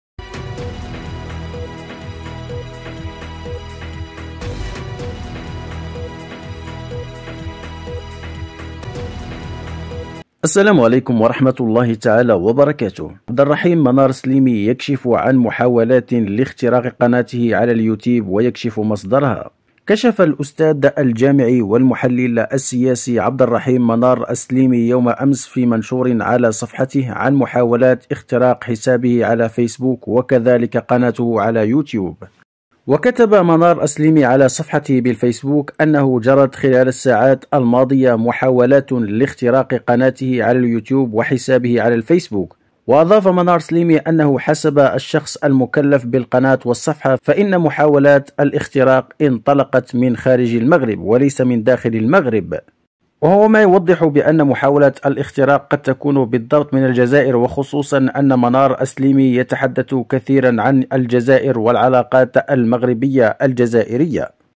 منار السليمي يكشف عن محاول سرقة قناته على يوتيوب.. وهذا مصدرها
منار السليمي يكشف عن محاولو سرقة قناته يوتيوب.. وهذا مصدرها